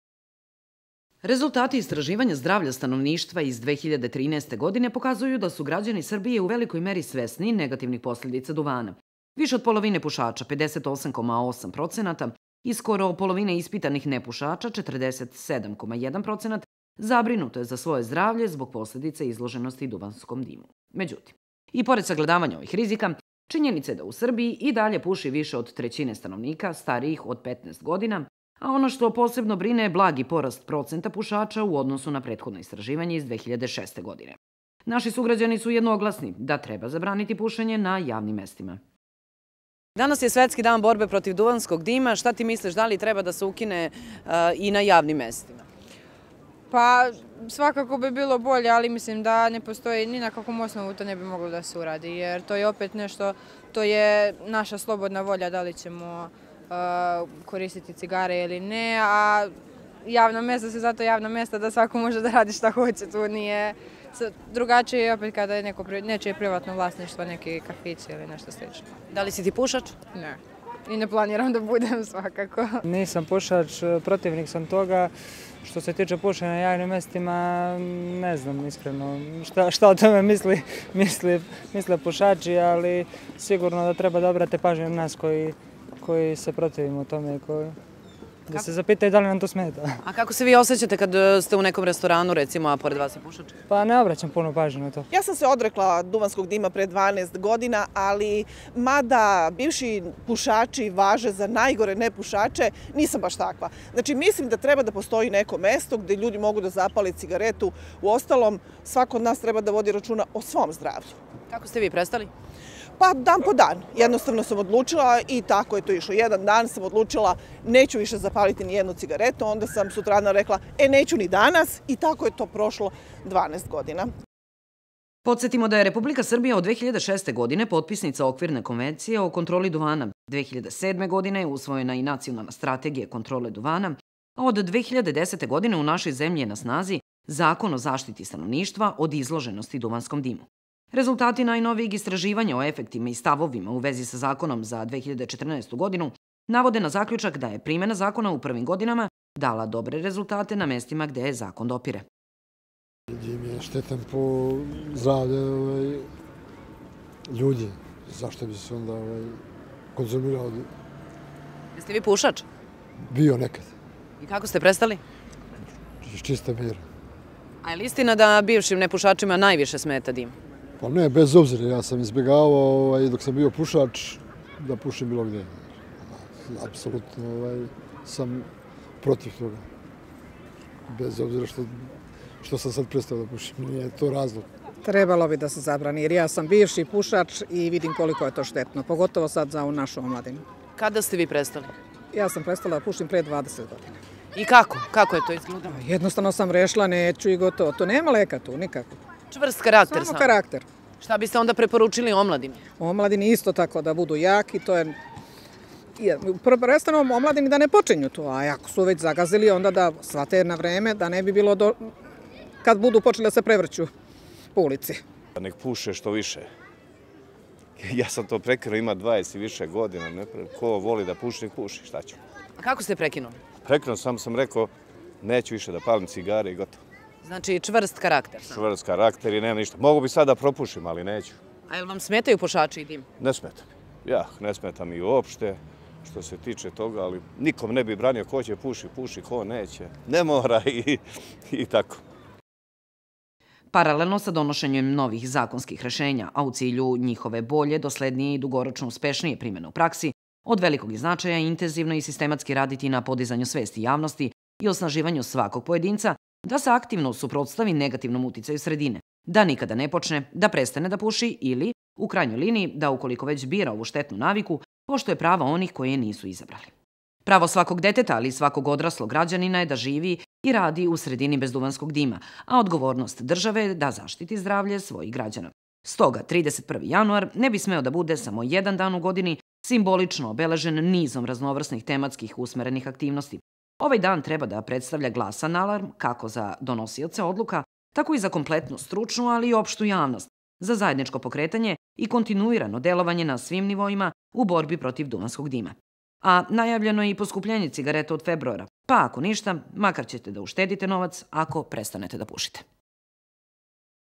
31 01 2020 NACIONALNI DAN BEZ DEUANSKOG DIMA
Nacionalni dan bez duvanskog dima u našoj zemlji tradicionalno se obeležava 31. januara i predstavlja simbol borbe protiv pušenja kao velikog globalnog i nacionalnog javnozdravstvenog problema. Naši sugrađani su uglavnom za to da pušenje treba da se zabrani na javnim mestima, ali ima i onih koji kažu da, iako su nepušači, duvanski dim im ne smeta.